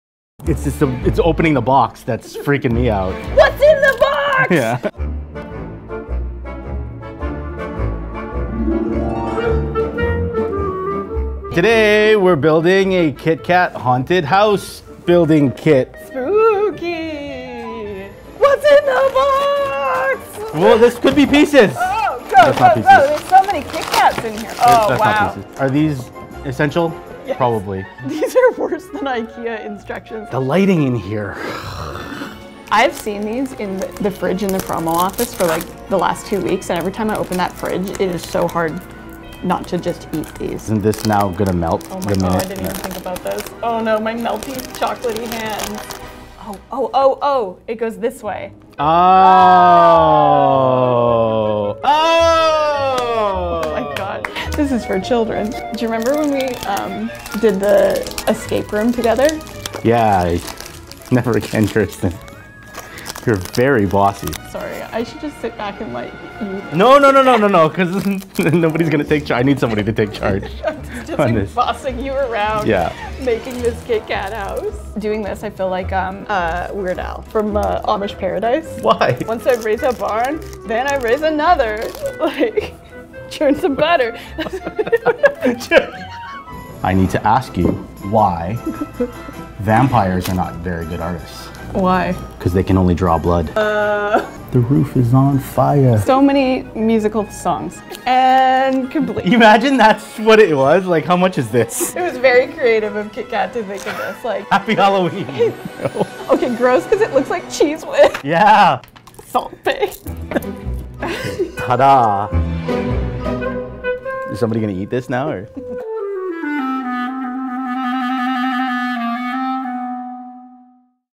Kit Katastrophe with Kirsten & Bryan
We bought three Kit Kat haunted house building kits and set our announcers free with a task a 4 year-old could complete. The results...well.